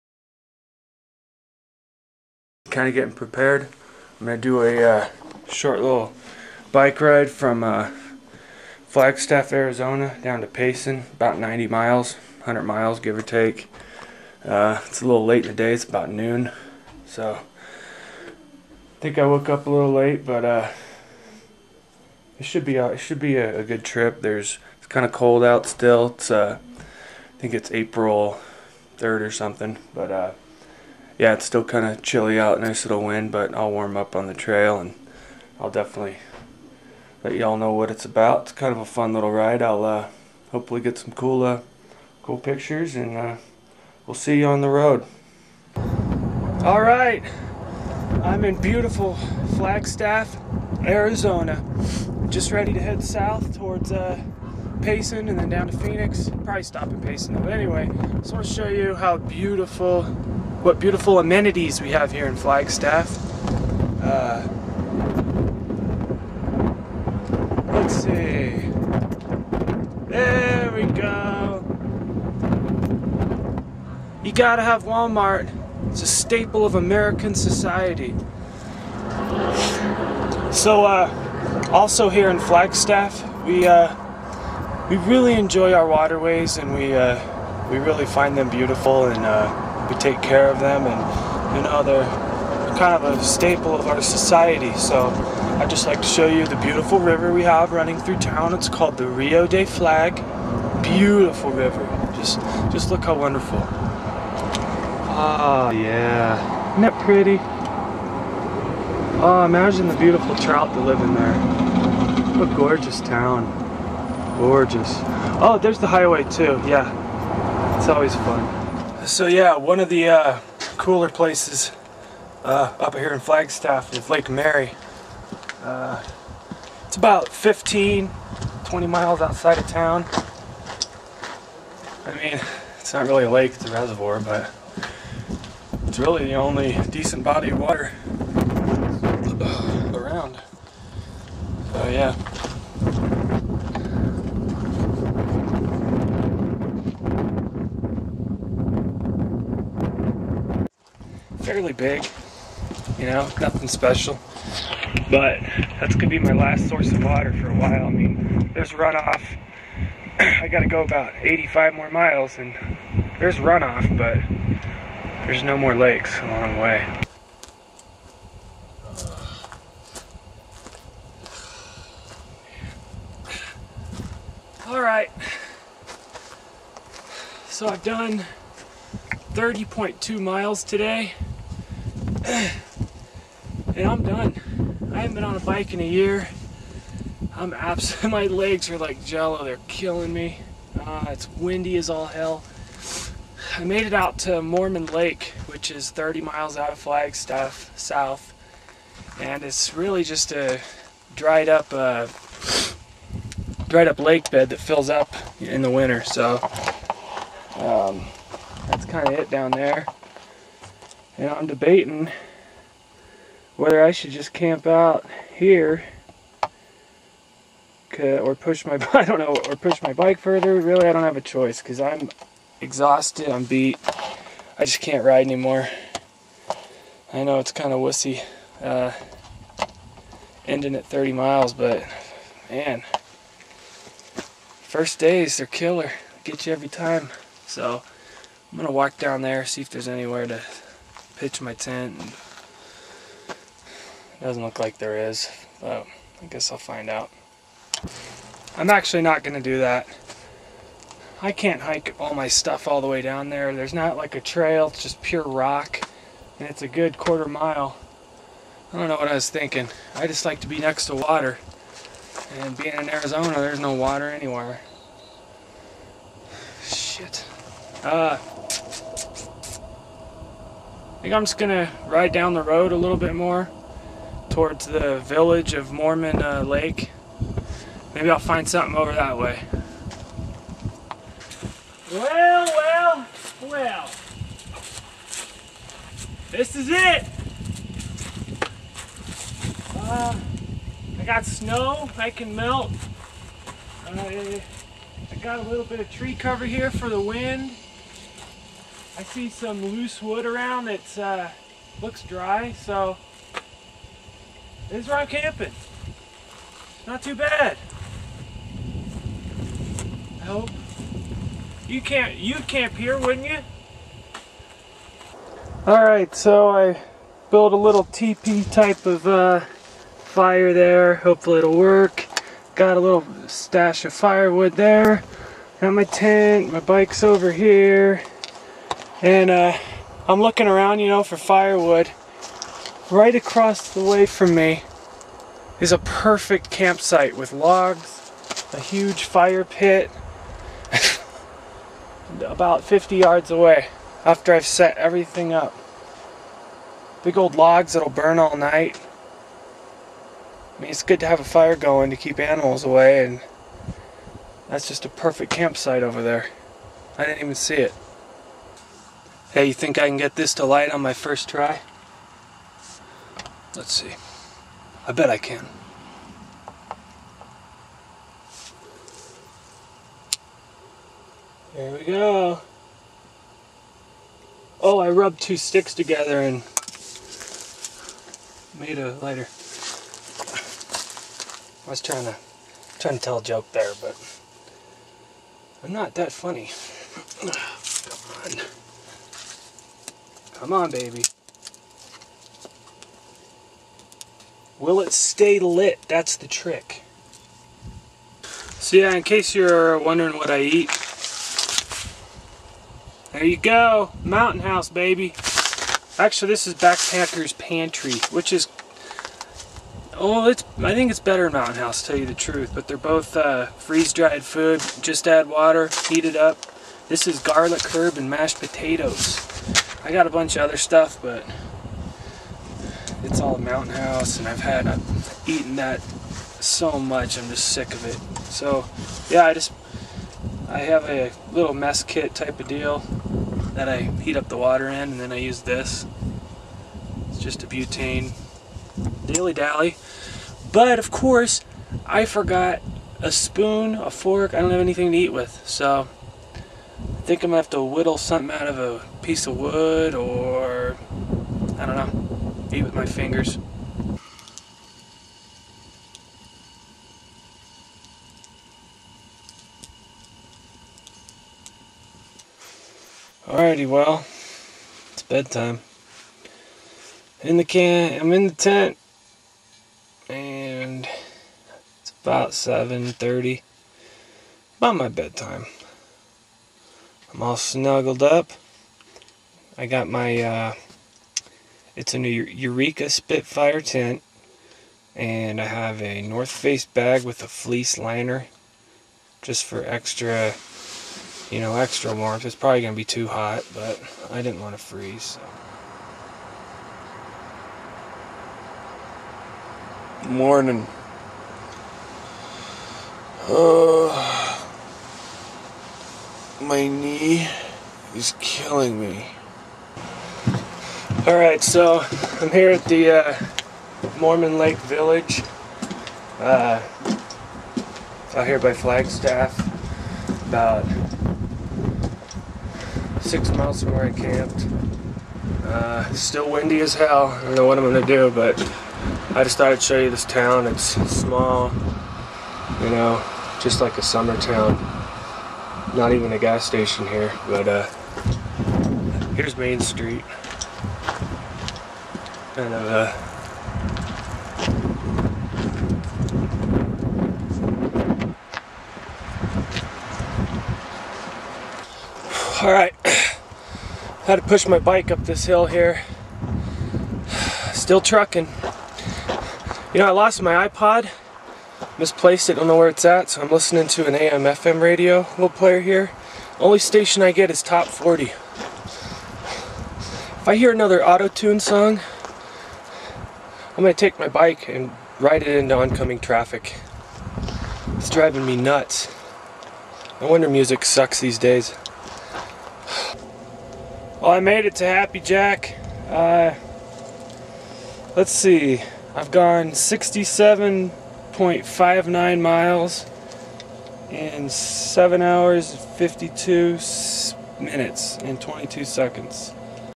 Bike Travel in Flagstaff
DiG rides his bike from Flagstaff to Clints Well.